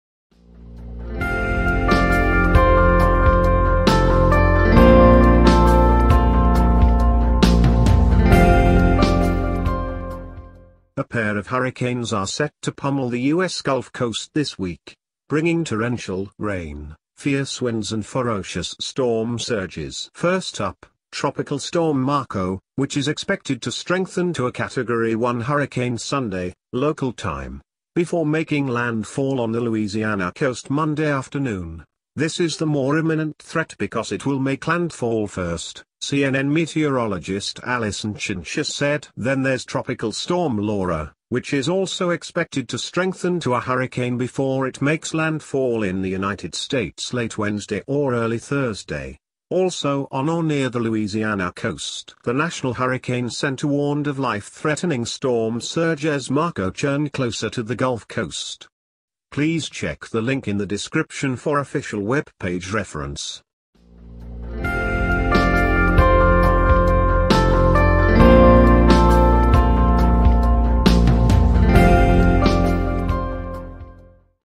Two hurricanes set to descend on US Gulf Coast
Then there's Tropical Storm Laura, which is also expected to strengthen to a hurricane before it makes landfall in the United States late Wednesday or early Thursday — also on or near the Louisiana coast.